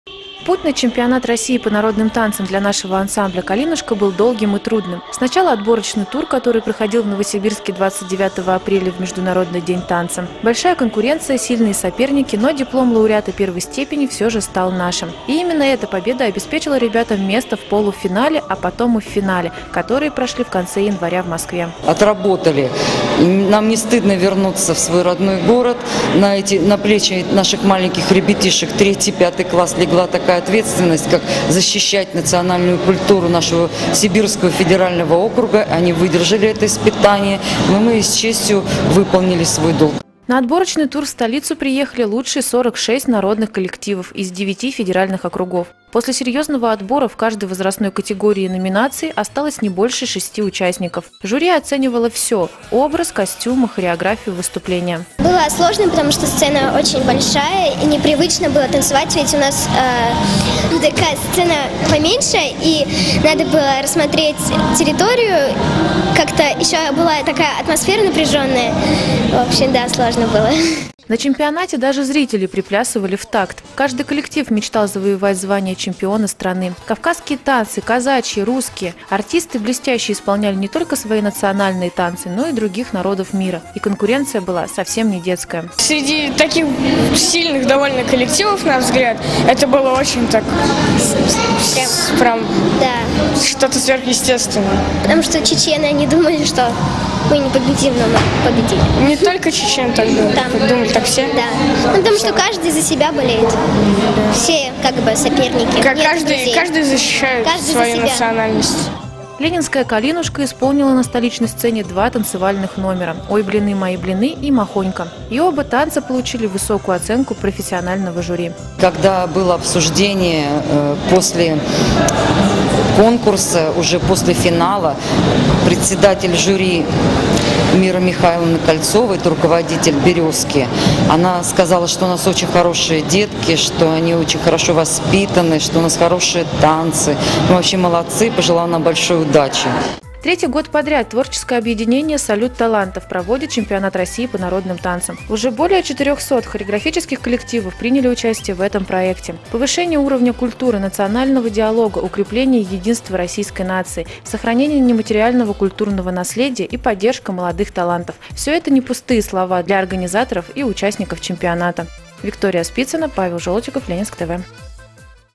Ансамбль «Калинушка» - лауреат Чемпионата России по народным танцам
В Москве прошел Чемпионат России по народным танцам среди детей. Зрители увидели весь спектр фольклора: от лезгинки и «Яблочка» до «Березки» и русского народного танца. Кузбасс на этих соревнованиях успешно представил ленинск-кузнецкий образцовый самодеятельный коллектив ансамбль народного танца «Калинушка».